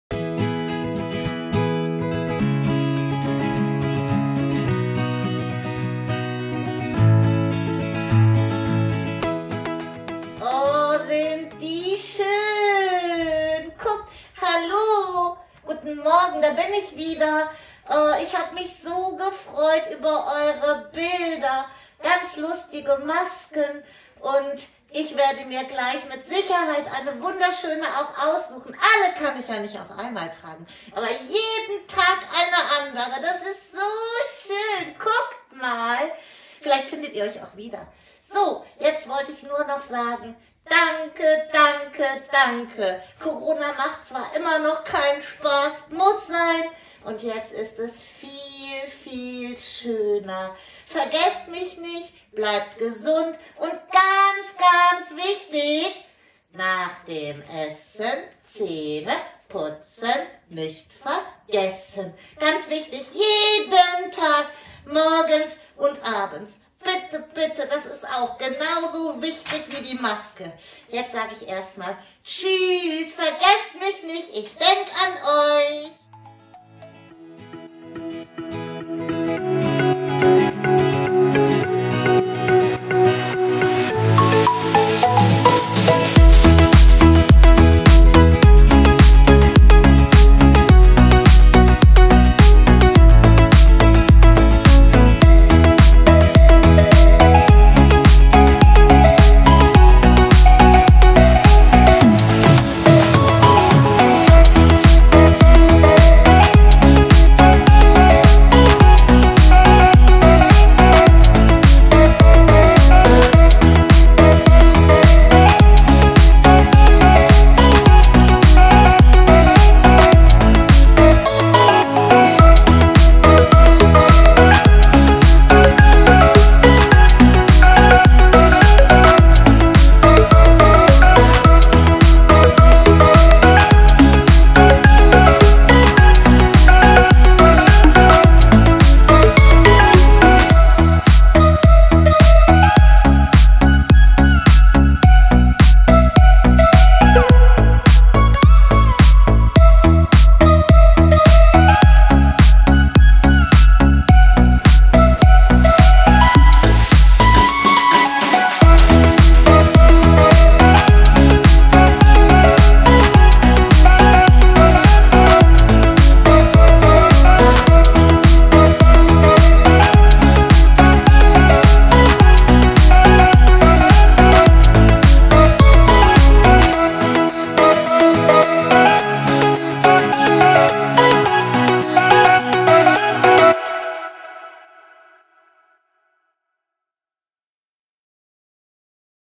10.6.2021: Zahnputzkrokodil Micki grüßt alle Kinder
Unsere Distanzschulung nähert sich dem Ende und hier folgt nun unser letzter Schritt: Der Micki-Gruß an alle Kinder.
Vielen Dank an alle, die mitgemacht haben und alle anderen, denen wir in dieser besonderen Zeit hoffentlich weiterhin ein Begleiter sein durften.
Bleibt alle gesund, wir sehen uns bald wieder und nicht vergessen: 3x täglich Zähne putzen!

Euer Zahnärztlicher Dienst des Kreisgesundheitsamtes Mettmann und die Arbeitsgemeinschaft Zahngesundheit.